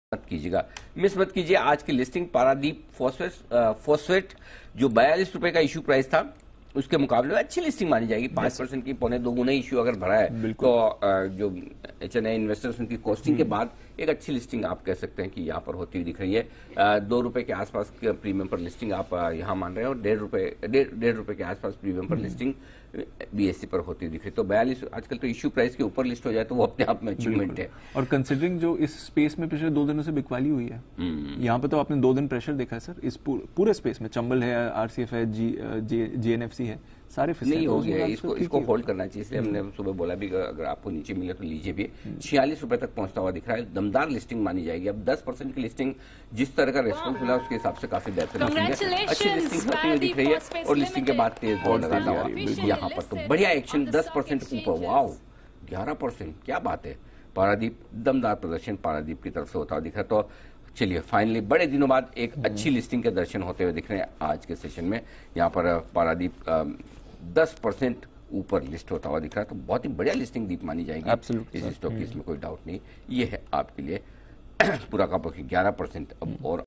Paradeep IPO debuted at Rs 44 on exchanges; up 4.7% from issue price of Rs 42/ share
ParadeepPhosphates की लिस्टिंग..

✨BSE पर ₹43.55 पर लिस्ट, इश्यू प्राइस ₹42
✨NSE पर ₹44 पर लिस्ट, इश्यू प्राइस ₹42